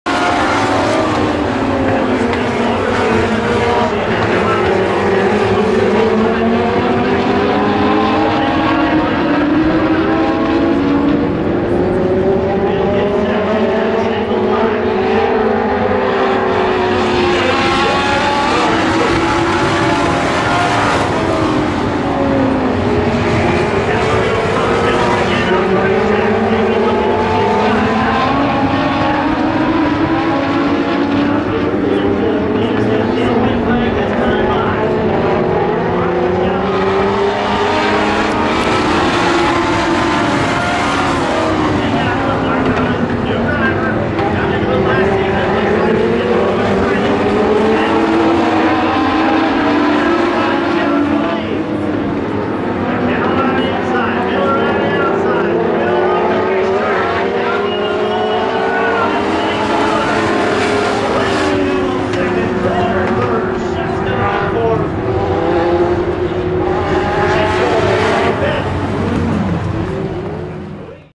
Beaver Dam '12 - Heartbreaker
The 00 and the 2M Legend cars duke it out for the top spot at Beaver Dam Raceway on April 21, 2012.  The 00 spins out in the final turn giving up the lead to the 2M.